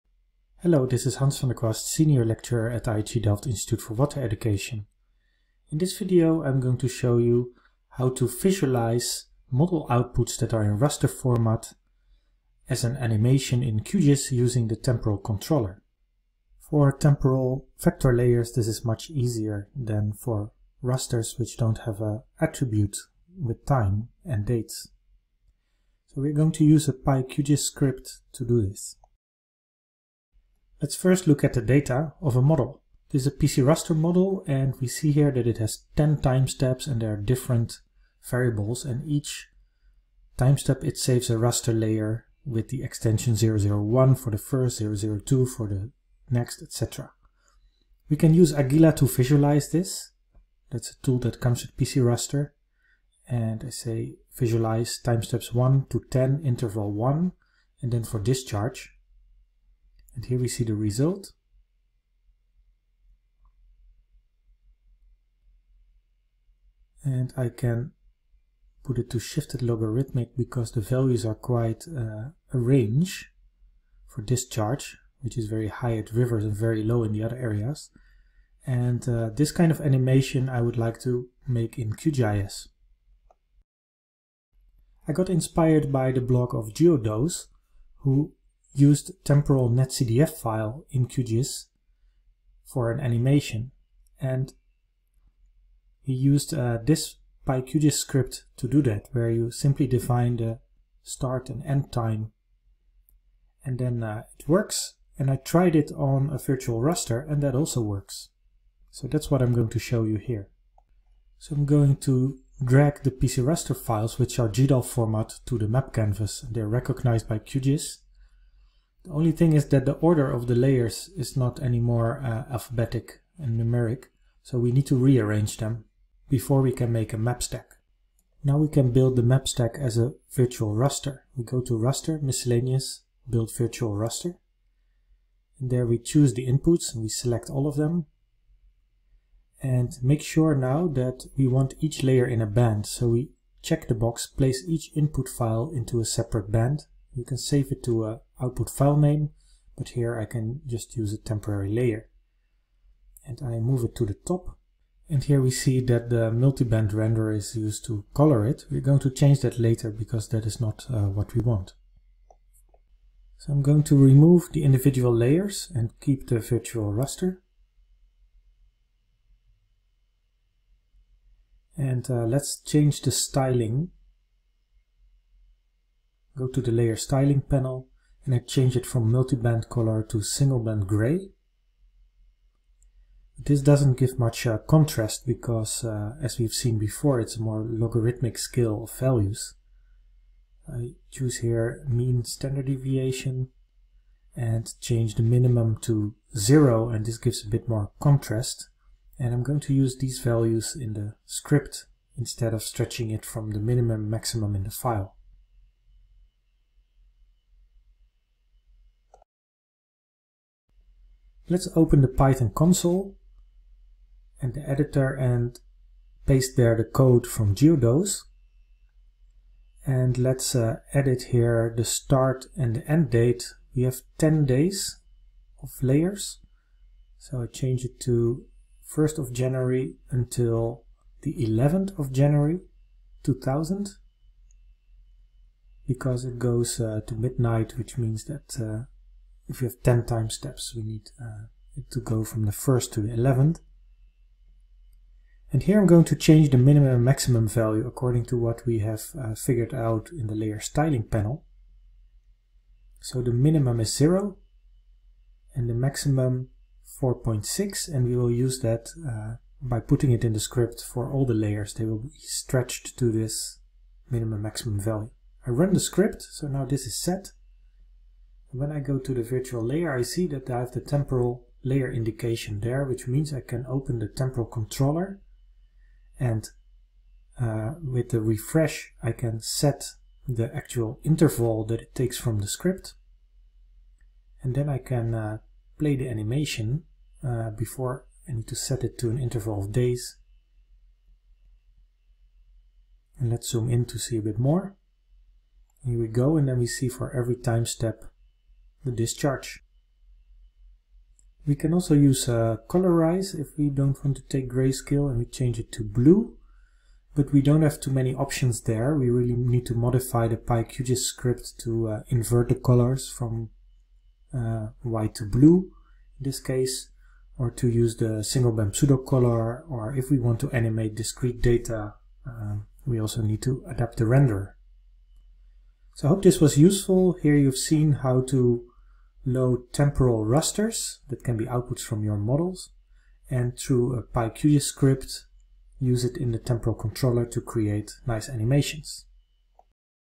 Visualise temporal raster model outputs in QGIS using the temporal controller
This video shows how to visualise temporal raster outputs of models in QGIS using the temporal controller.
We will visualise PCRaster temporal rasters.
The method is based on this blog by Geodose for visualising NetCDF temporal data:

In the blog you find the script that is used in the video.